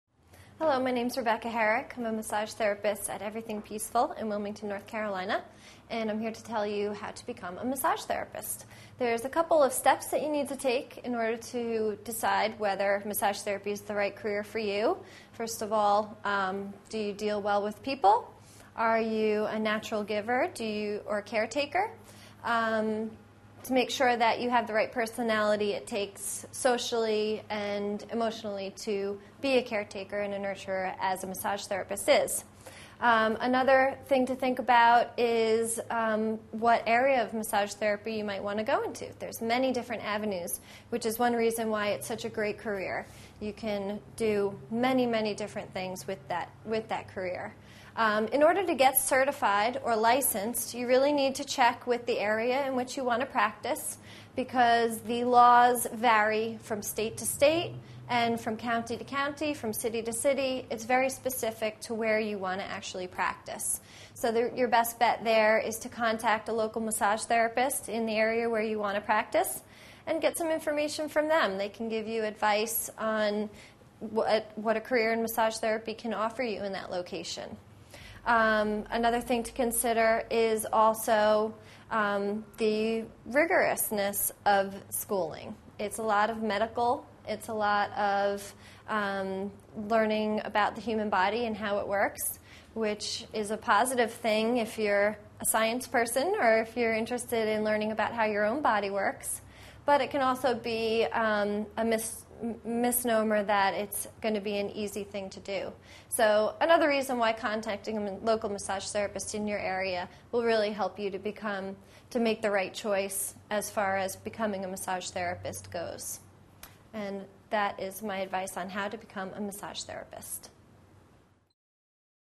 How to Become a Massage Therapist
How to Become a Massage Therapist. Part of the series: Massage Therapy. Before becoming a massage therapist, a person should decide if they have the right personality to be a social and emotional caregiver. Become a massage therapist with tips from a licensed massage therapist in this free video series on massage therapy.